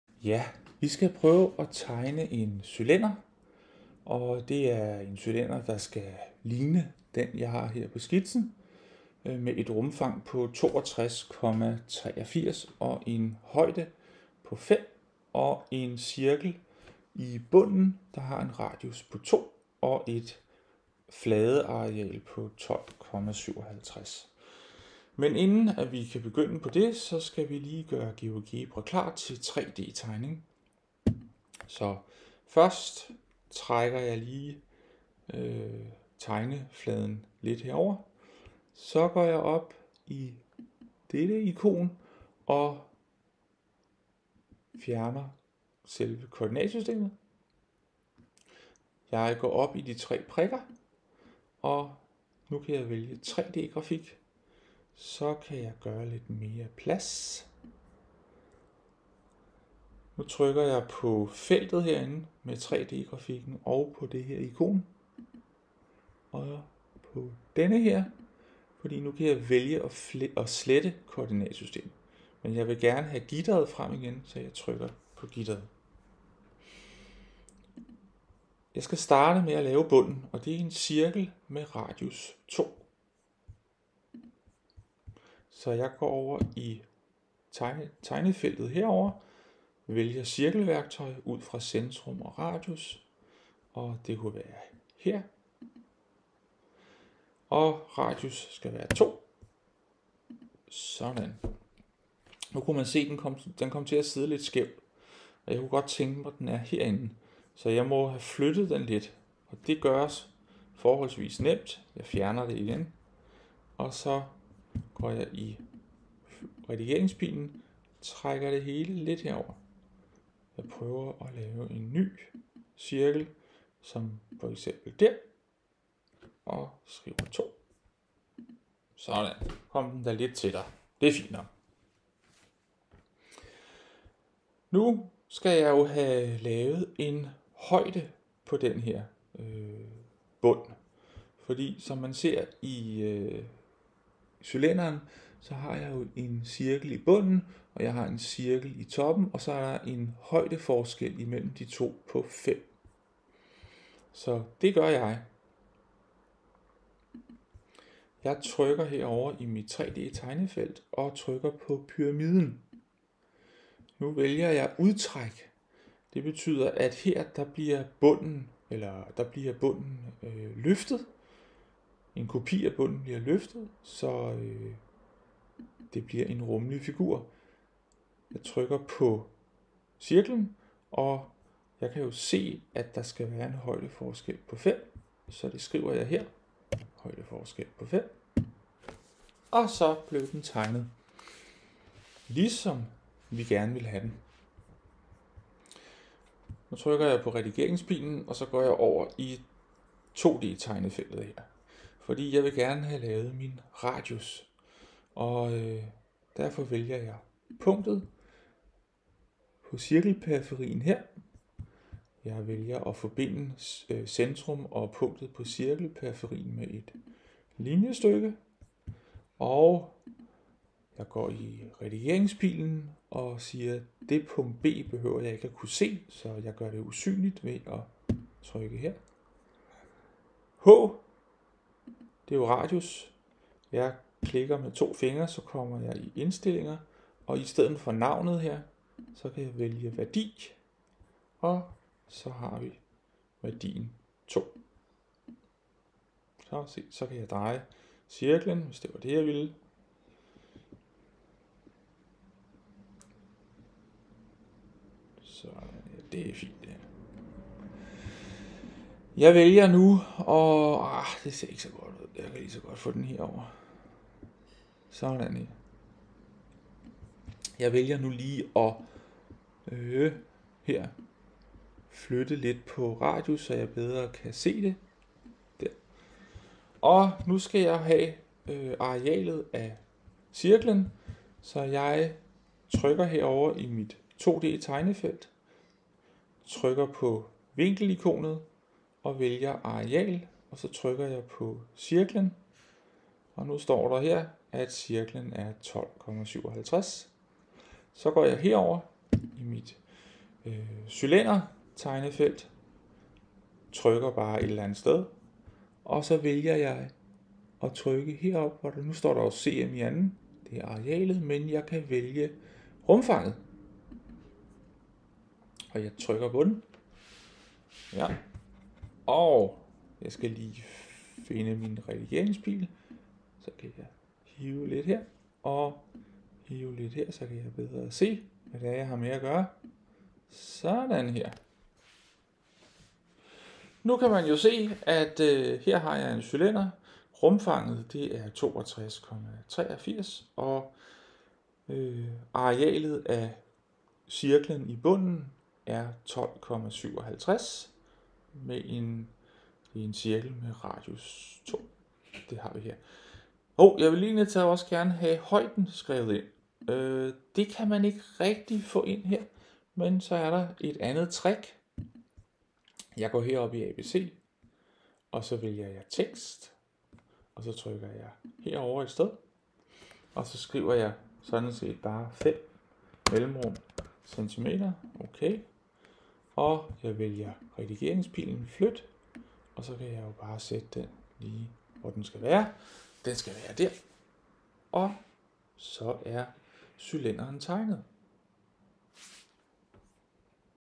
Geogebra og cylinder
tegn en cylinder i Geogebra og find rumfanget